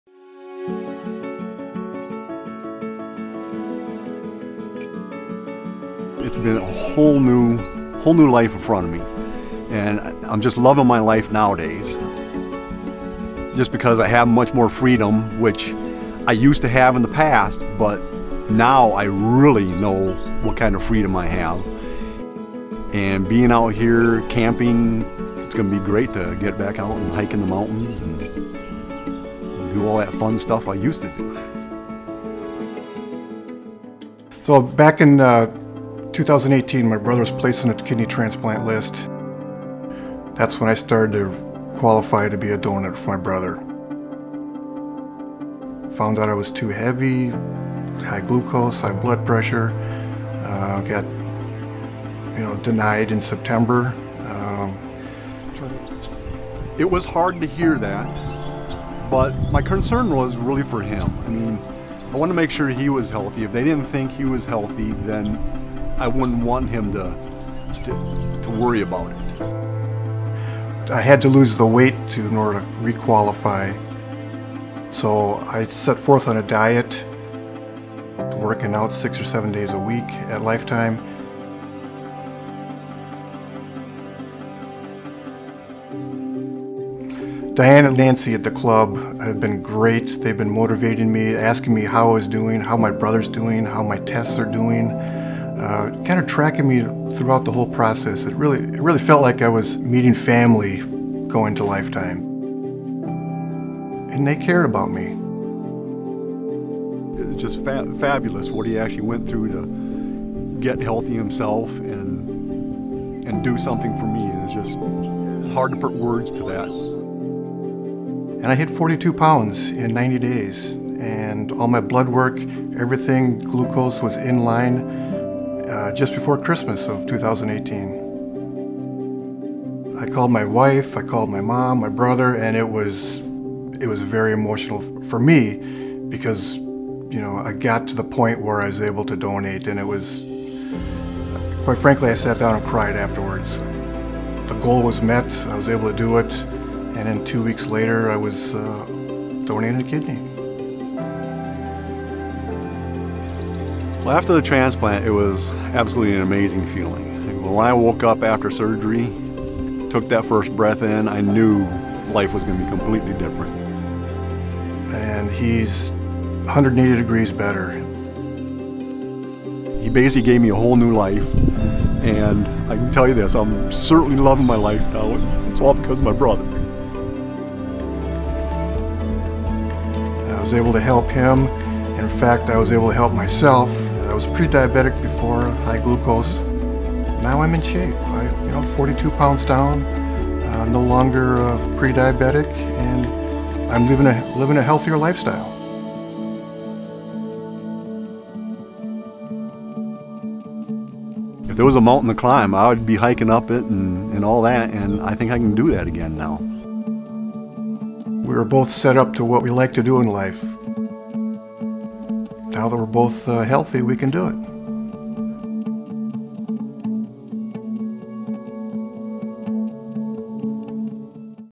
Scott Morton Member Story
When Jeff Morton needed a life-saving kidney transplant, his brother, Scott, volunteered to be his donor. But Scott’s own health issues made him ineligible. Already a Life Time member, Scott committed to improving his own health — and soon discovered that in saving his brother, he was also saving himself.